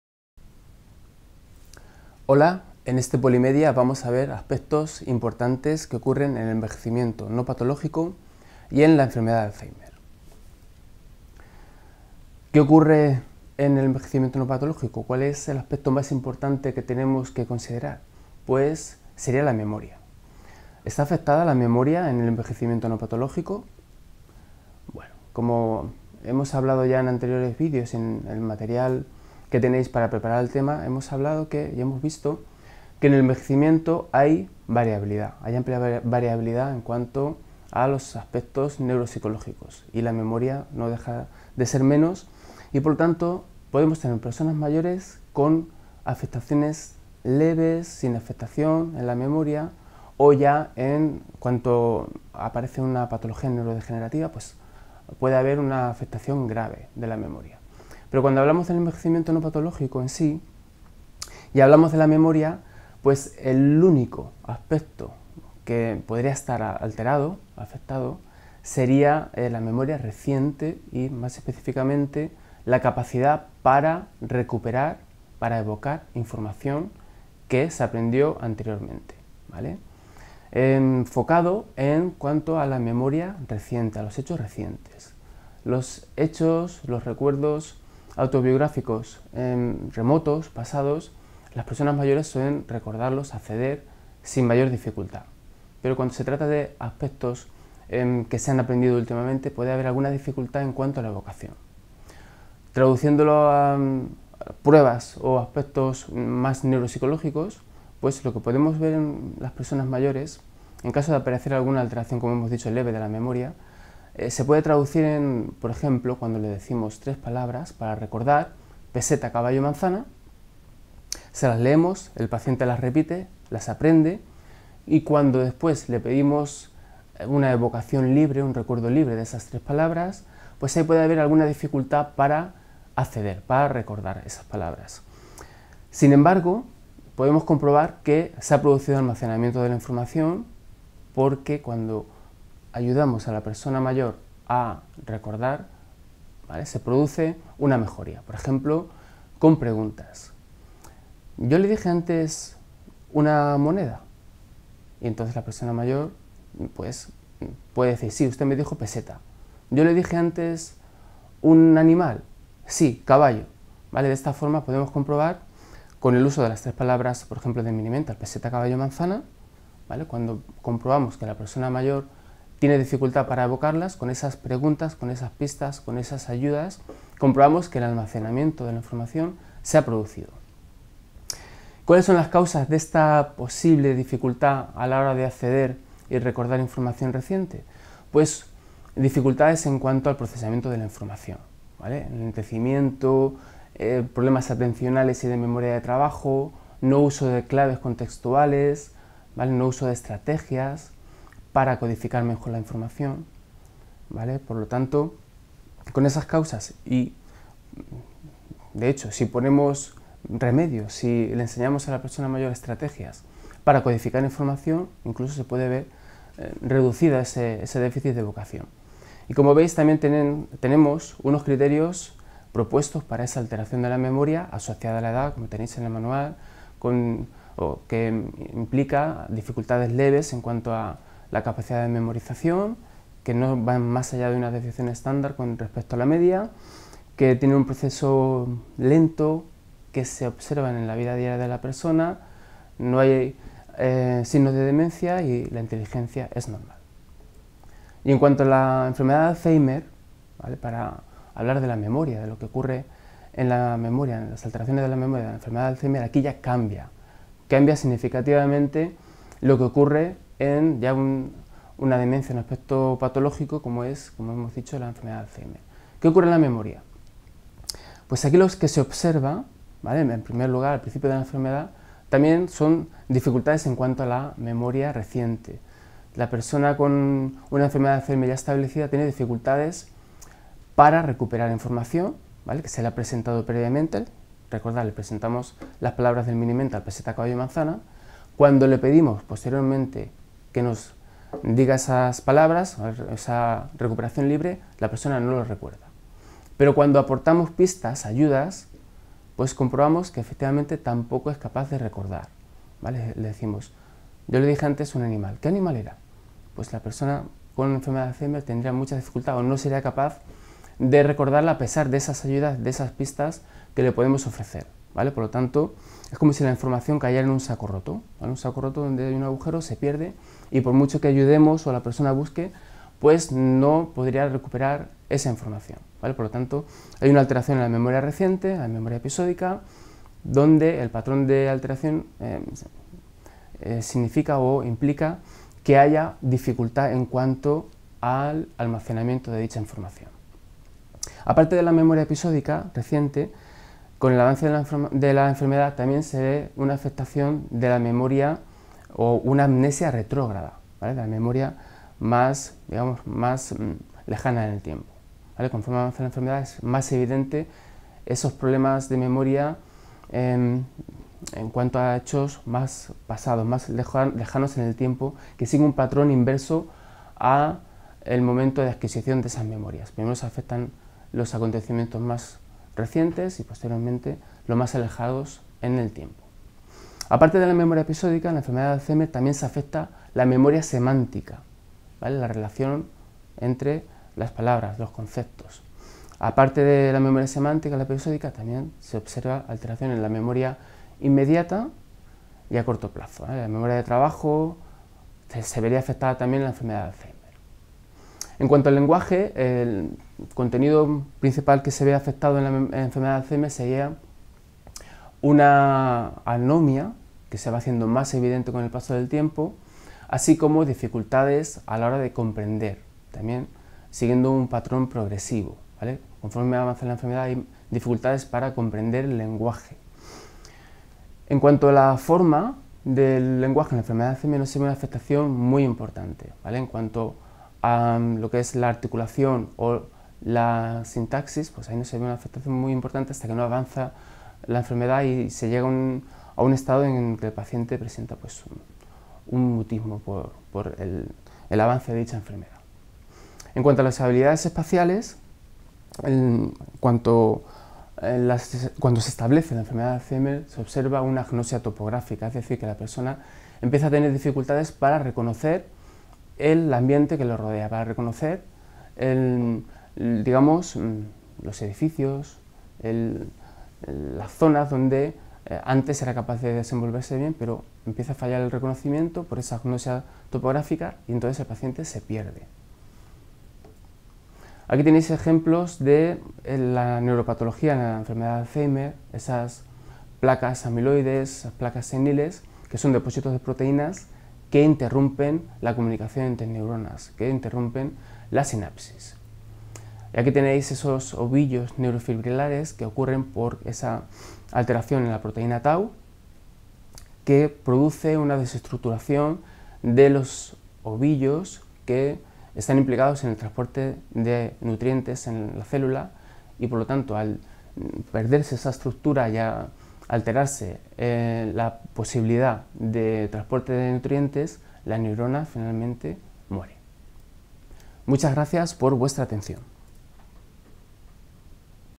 Neuropsicología - Tema 3. Envejecimiento y EA - Pedro Jesús Fernández
Universidad Católica de Murcia

Grado en psicología ONLINE